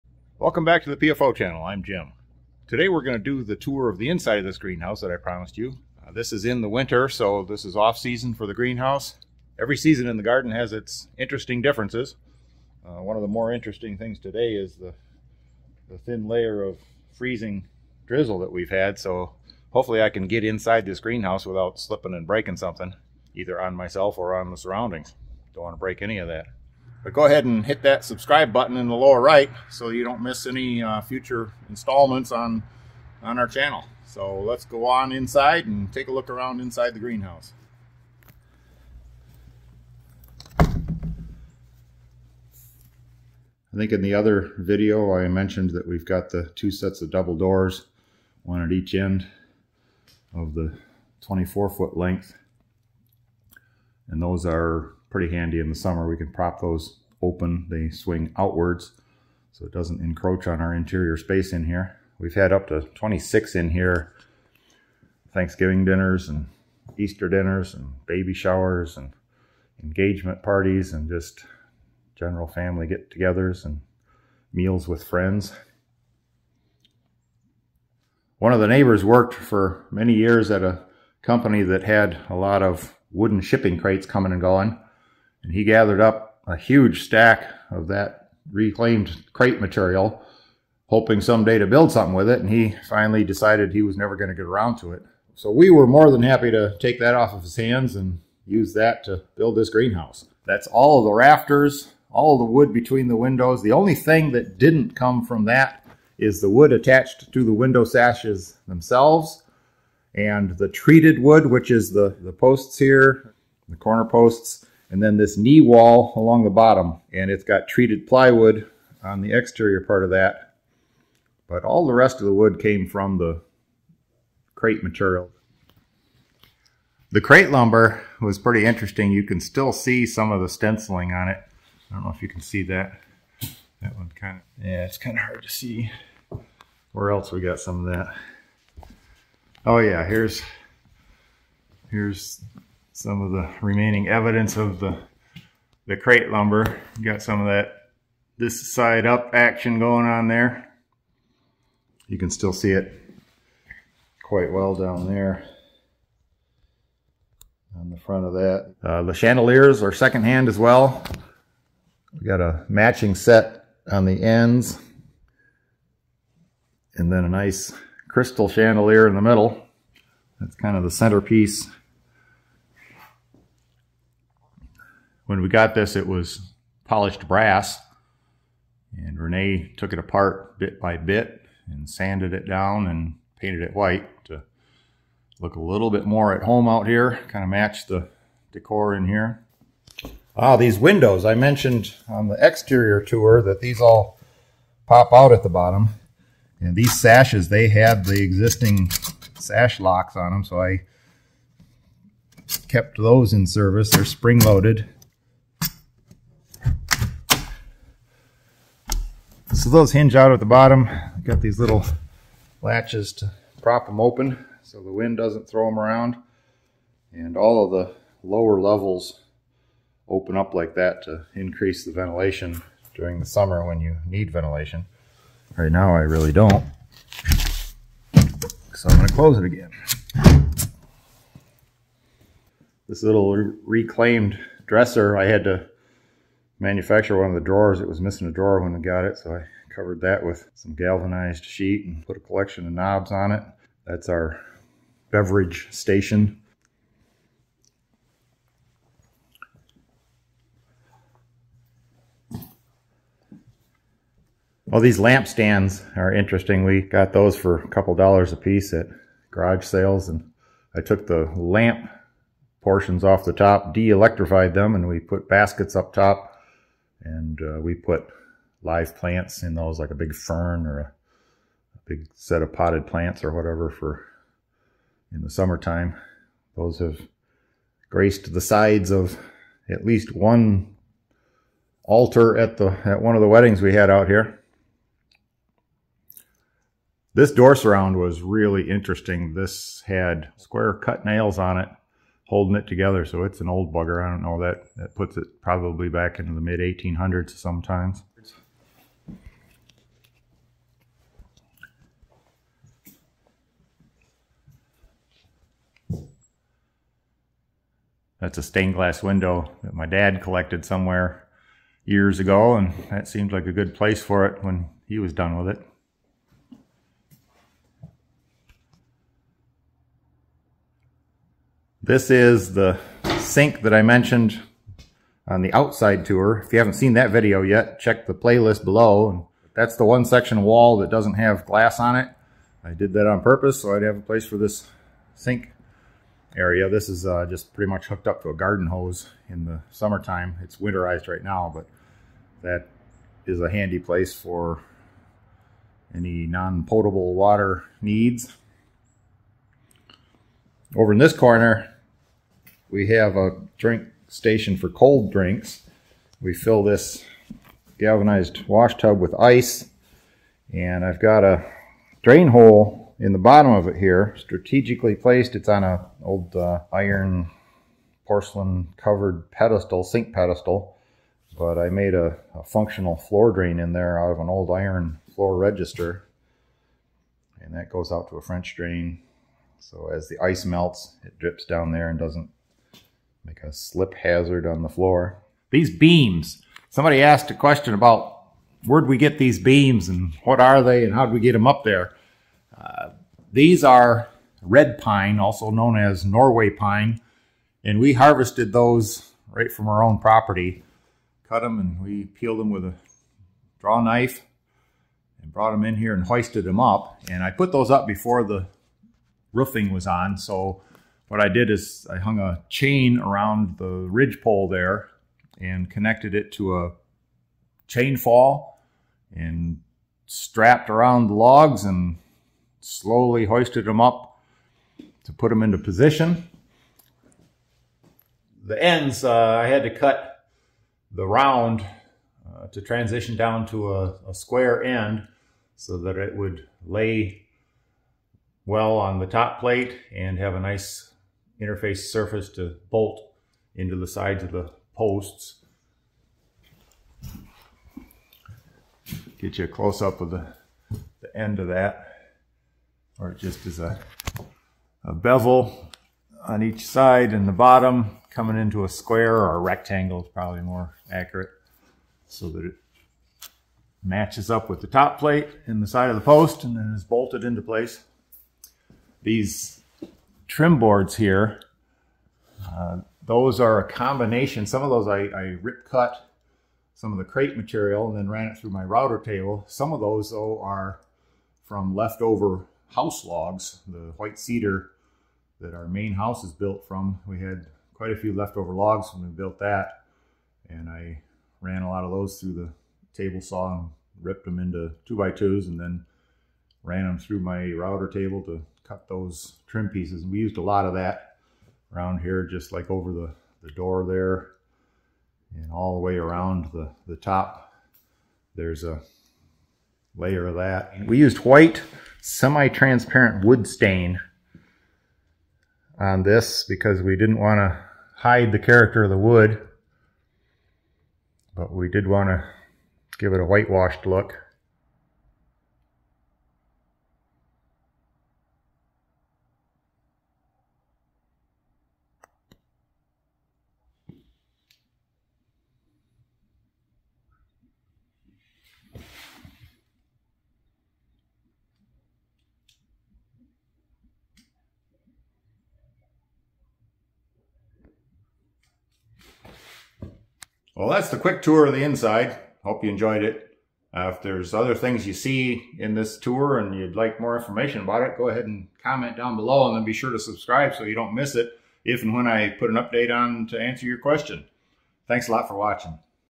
Transform Your Garden with a Stunning DIY Greenhouse
Step inside our one-of-a-kind DIY greenhouse crafted from reclaimed materials, including salvaged wood windows. Explore every nook and cranny of this sustainable creation in this interior walk-through. Check out the exterior tour in our playlist and let us know if you'd like a closer look at any specific details.

Thanks to @RiverForged on IG for help with the thumbnails!

00:00 - Start
00:43 - The Table
01:18 - Reclaimed Shipping Crate Lumber
02:38 - Reclaimed Chandeliers
03:09 - Lower windows open for ventilation
03:53 - Beverage Station-Reclaimed Dresser
04:14 - Plant Stands from Reclaimed Floor Lamps
04:46 - Oval Topped Door Surround
05:08 - Stained Glass Window
05:20 - Reclaimed Farmhouse Sink
05:48 - Cold Drinks Station
06:25 - Beams from Local Pine Logs
07:48 - Wood Moldings
08:30 - White Stain